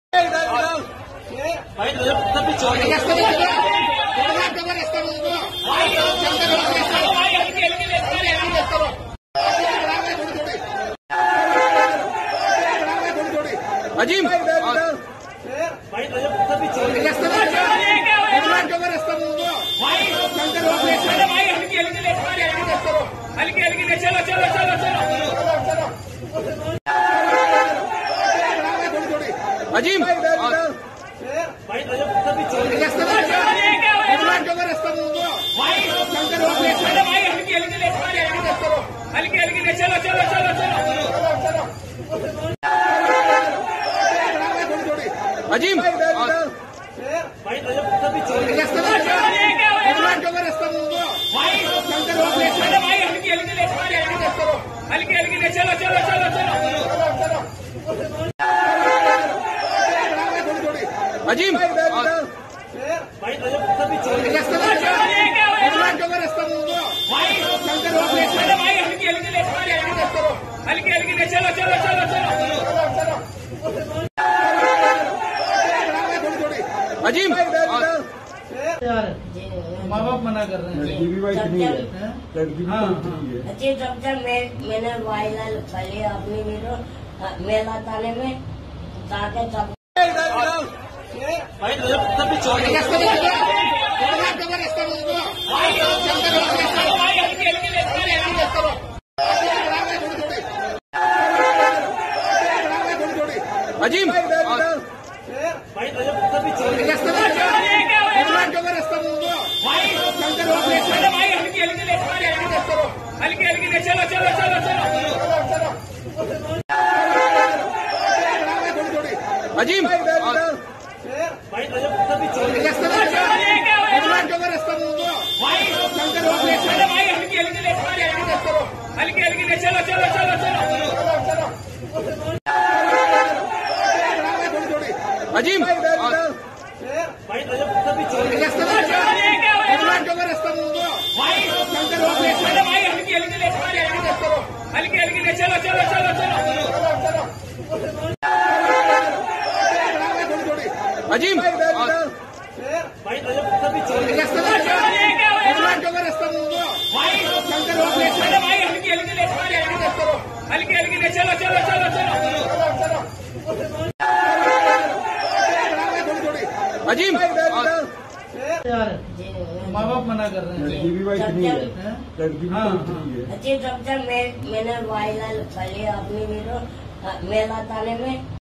2.3 FEET KE SHAKS AZEEM MANSURI JO POLICE STATION PAHUNCH GAYE THE DULHAN NA MILANE PER SHADI HOGAI
2.3 FEET KE SHAKS AZEEM MANSURI JO POLICE STATION PAHUNCH GAYE THE DULHAN NA MILANE PER UNKI HUI SHADI UTTAR PARDESH